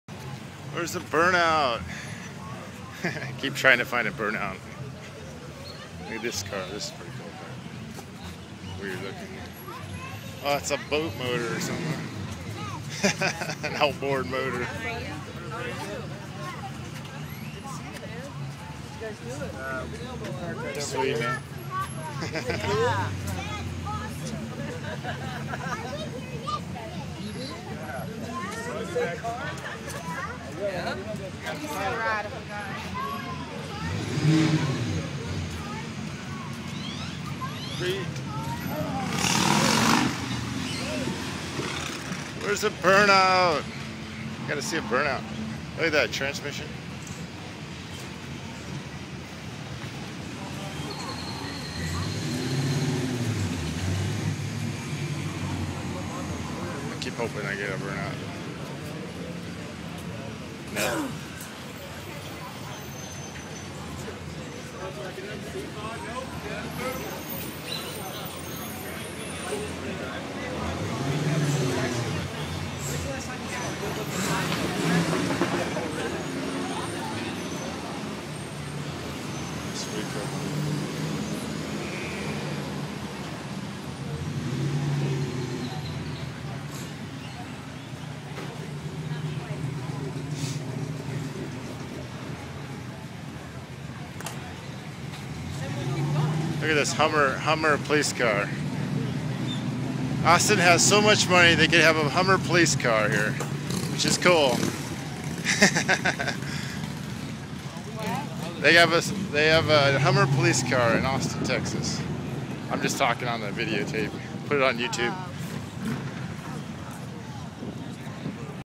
Car show Austin TX
Cool Cars parked on the street.. I got to build one someday. Still like the old ones also.